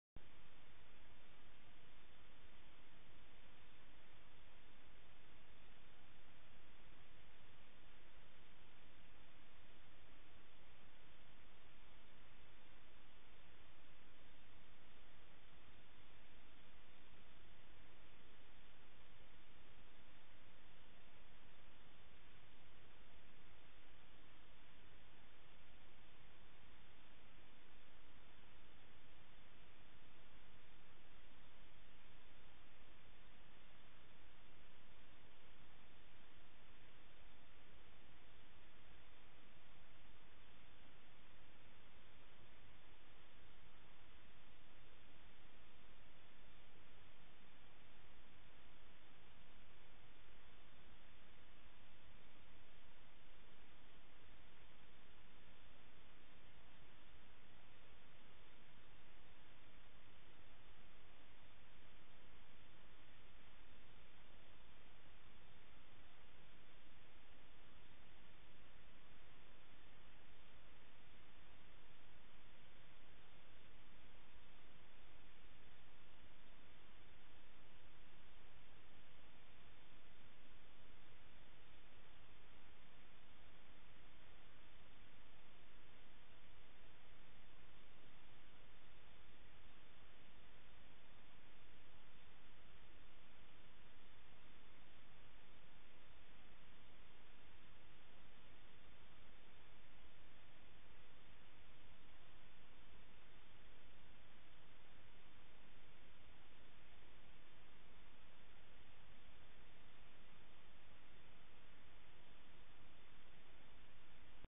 Roubo de cabo de telefone no bairro Nova Granada
Câmera de segurança de casa flagrou momento em que ladrão corta e leva cabo de telefonia.